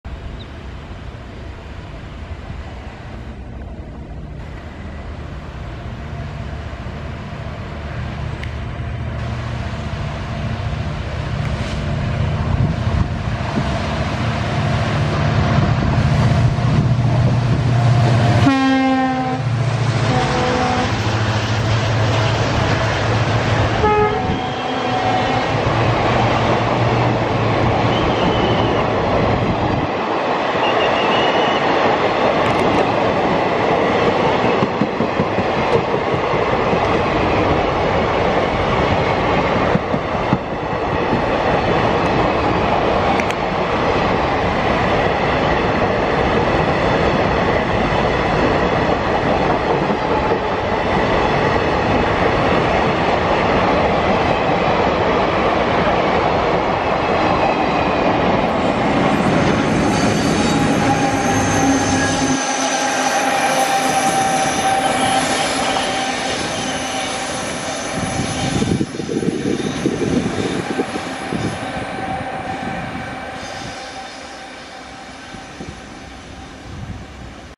COVID19 DAILY SPECIAL KONARK EXPRESS : CURVES & BEAUTIFULLY ENTER's MONKEY HILL TUNNEL : BHOR GHAT
Here Is Our First COVID19 Daily Special Konark Express From my Best Location

KALYAN WDP4D 40277 LEADS MUMBAI BHUBANESWAR KONARK EXPRESS